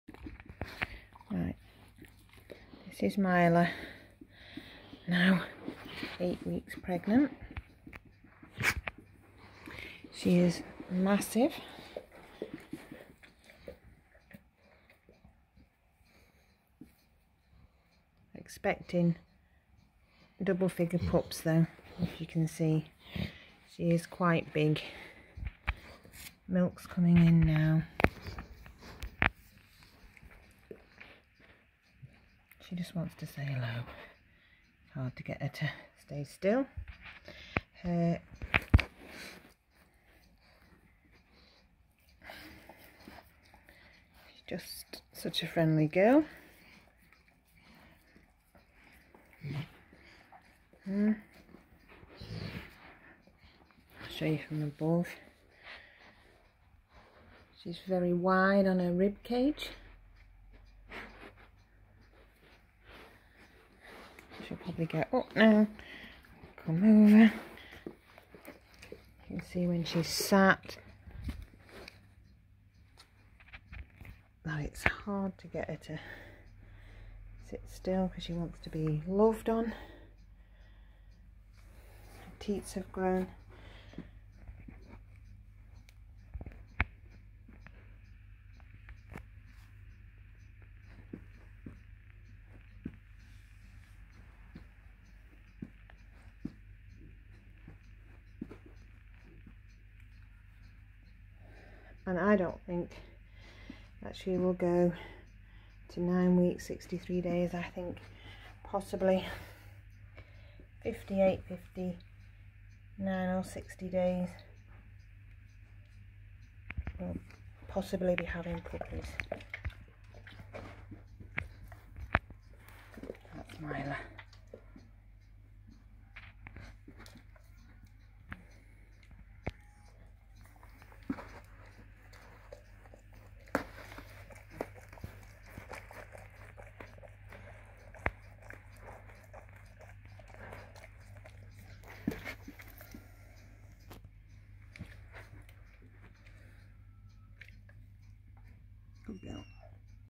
German pointer GSP 8 weeks pregnant
Myla is now 8 weeks pregnant and starting to sleep and rest more. she is very large considering she is normally so slim snd ultra fit.
she still wants to run when outside so we have to be careful.. you can't switch a GSP brain off!
she caught a pigeon a few days ago and brought it to us.